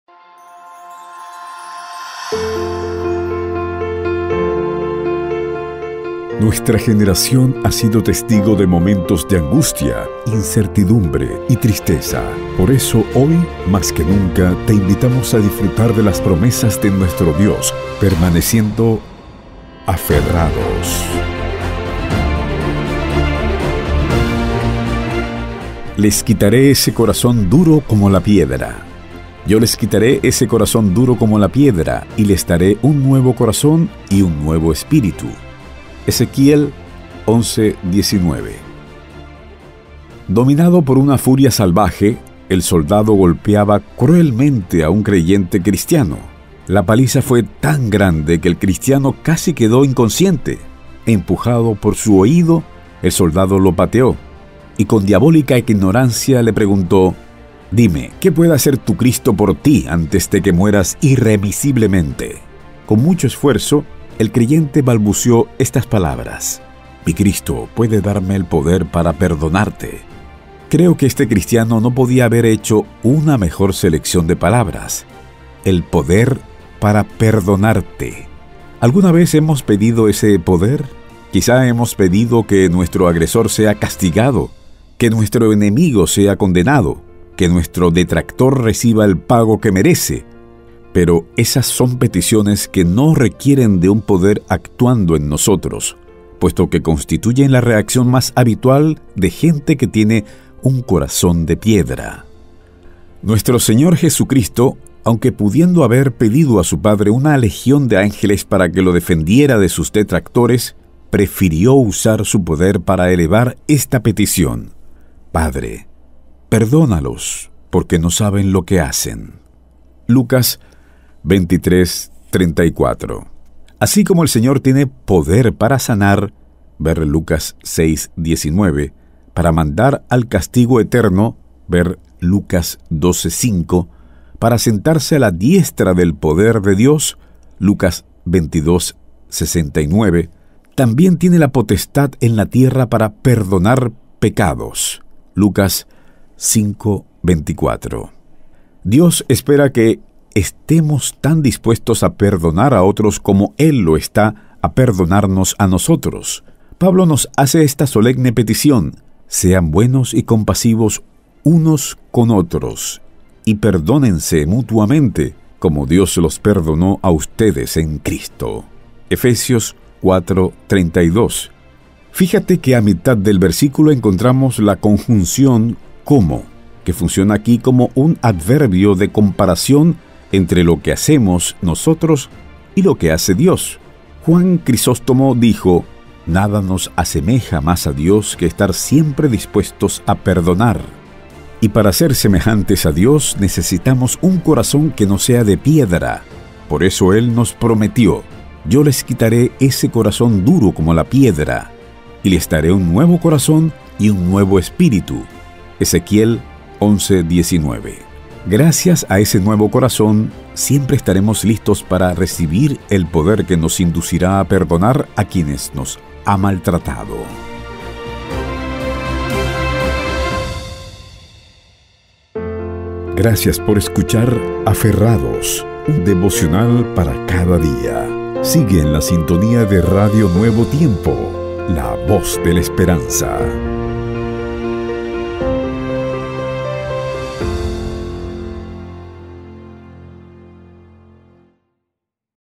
DEVOCIÓN MATUTINA | ADULTOS 27 DE MAYO | "LES QUITARÉ ESE CORAZÓN DURO COMO PIEDRA" - AFERRADOS
✨Dios espera que estemos tan dispuestos a perdonar a otros como él lo está a perdonarnos a nosotros. Para ello promete darnos un nuevo corazón si así lo queremos. Gracias a ello, siempre estaremos listos para recibir el poder que nos inducirá a perdonar a quien nos ha maltratado. 🙌🏼❤️

📖 Lee para hoy: Ezequiel 11:19